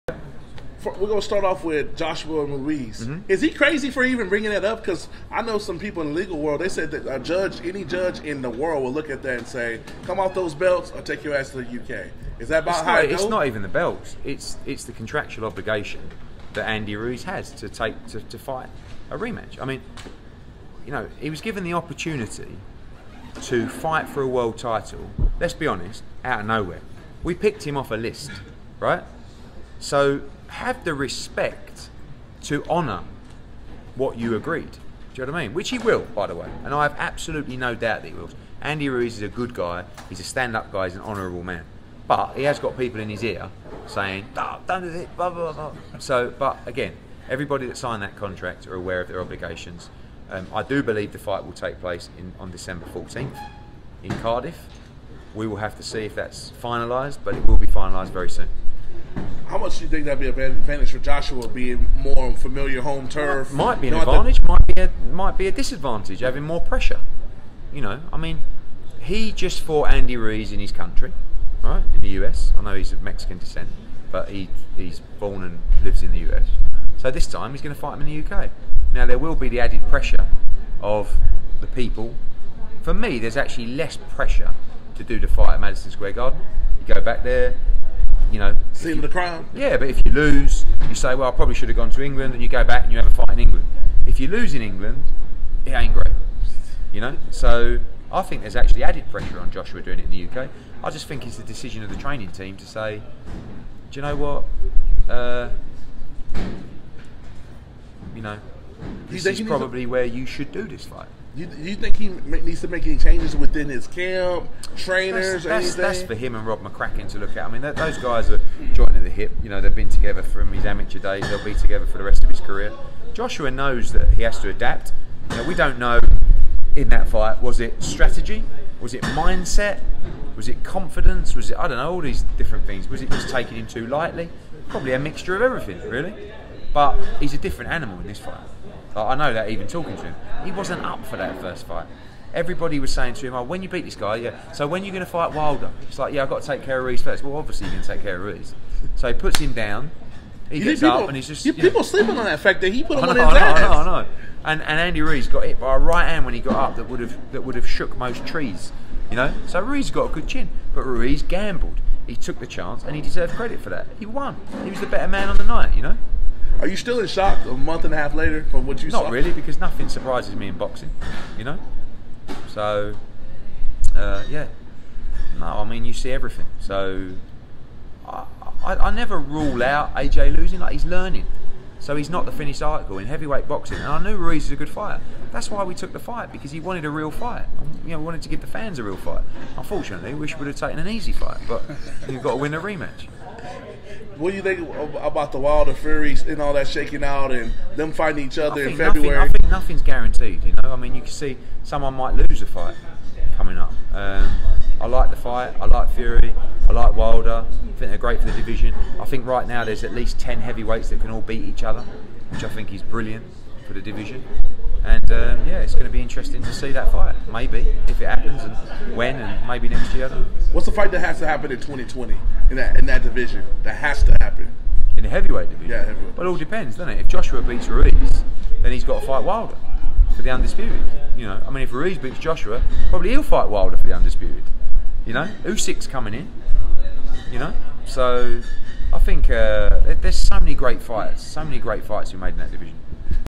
Eddie Hearn Responds Andy Ruiz Refusing Anthony Joshua Fight In UK: Have Respect & Honor Contract!
Eddie Hearn on Andy Ruiz Refusing Anthony Joshua Fight In UK: Have Respect & Honor Contract!